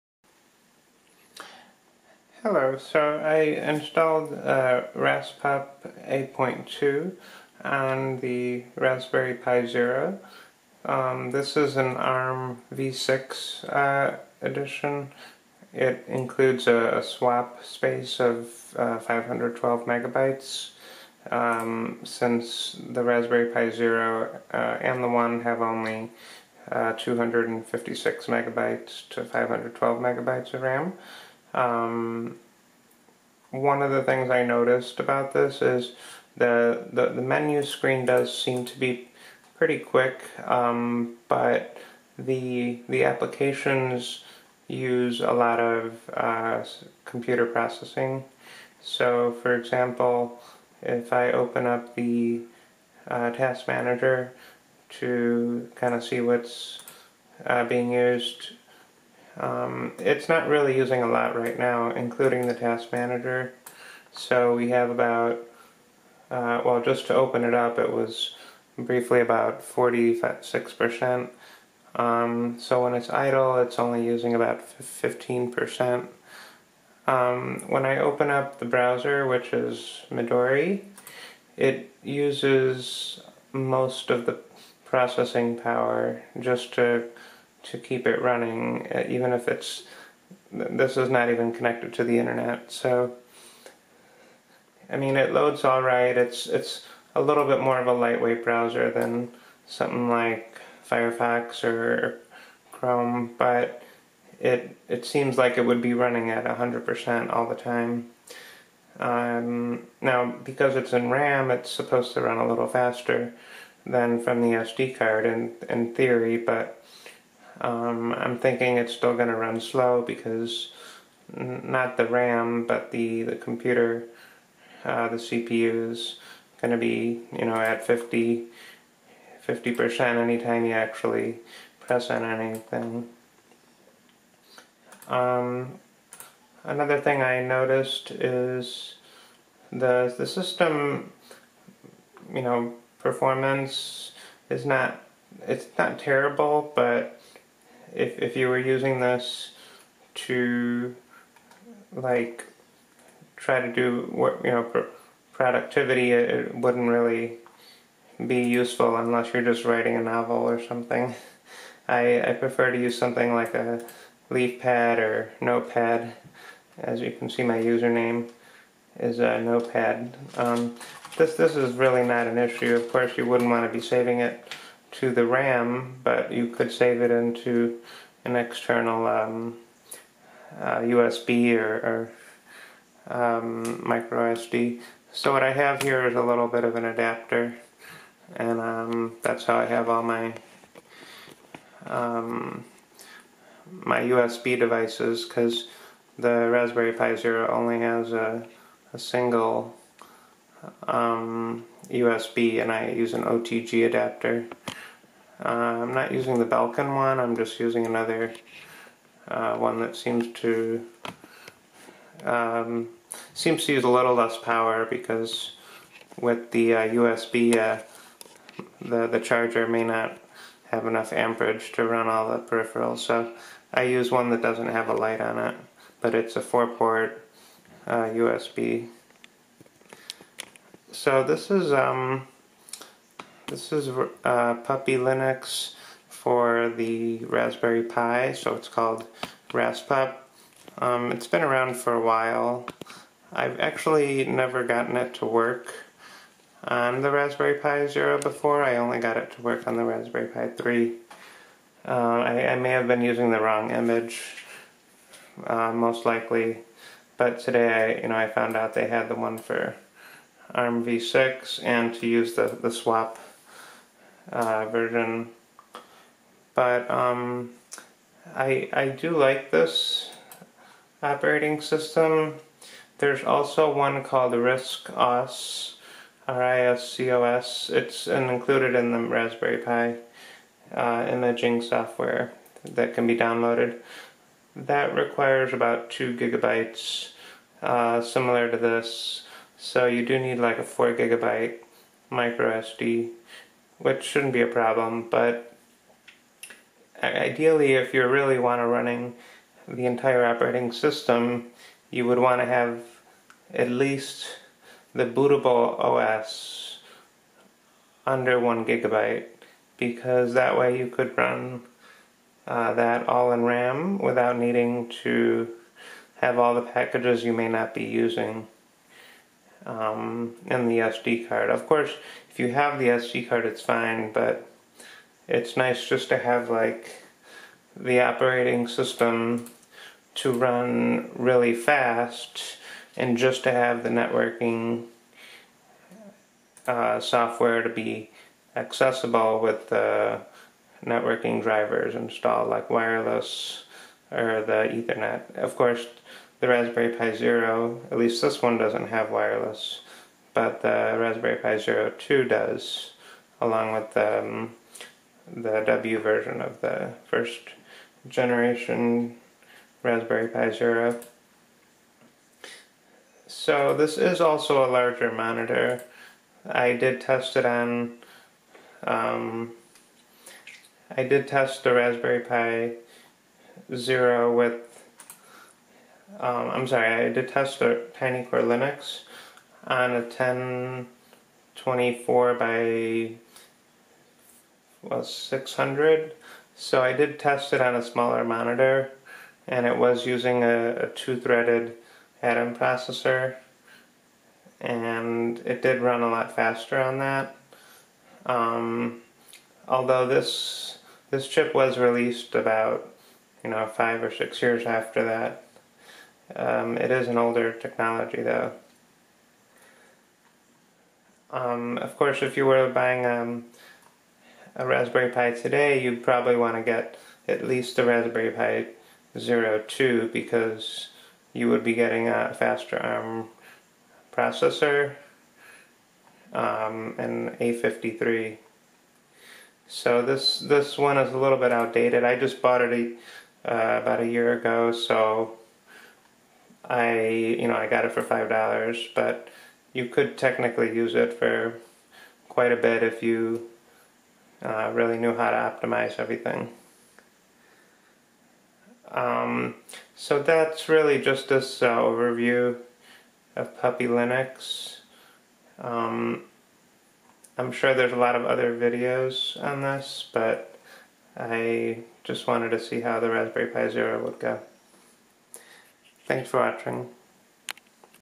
Raspup 8.2.1 on Raspberry Pi Zero (armv6)
Running Raspup  (Puppy Linux for Raspberry Pi) on my Raspberry Pi Zero

I used Balena Etcher to write to micro SD card

Review: The OS boots to RAM, like all other Puppy Linux live distros. But it takes a while to load the image, I lost count of the minutes but over 5 and less than 15. Running anything other than a text editor uses more than 50% of the CPU utilization. Midori takes a while to load. Overall, it's better than nothing, but not as snappy as Tiny Core linux- athough  that didn't have any applications preinstalled.

Ideally there is a puppy linux distro that has none of non-essential apps/drivers pre-installed, except for essential I/O (mouse, keyboard, display, and network hardware like wifi and lan) so more modules can be downloaded (or accessed via microSD) by hyperlink/pac manager for quick access. Minimum SD card size for image is 2GB! Why not offload it to SD and have the OS load far fewer drivers/modules...? Uncompressed image should be less than 256MB, imo. I did mistakenly mention the OS should be running under 1GB, which it wasn't running that much in RAM, but I was referring to the total image (on the SD- which isn't a storage issue, but a loading issue)

i bought mine over a year ago for $5. The Rasp Pi 3B + is a little nicer because it has ethernet and USB-A ports, along with full HDMI out.

If you don't need that and already have a mini-HDMI-to-HDMI cable and an OTG cable with a USB Hub, then the Pi Zero 2 is a slightly cheaper deal at $15.

Does this work better than the Raspberri Pi OS? I prefer Raspup running in RAM because it is faster than the stock OS, but I think the Raspberry Pi OS is a little more user friendly. If you know what all the apps are, this should not be too difficult (there are many other linux distros far more difficult).

For browsing without Javascript, Netsurf would be a much simpler browser to allow the CPU utilization under 75%. Midori being at 100% will cause delays even for casual browsing. Dillo is another fast browser but has even less modern support.

I've wanted to install Tiny Core Linux on the Rpi Zero, but I am not great with it, nor does mine have wifi, so I need to know some of the drivers. You can check out this video to see how well the performance looks compared to Raspup- around the 11:00 mark they load the desktop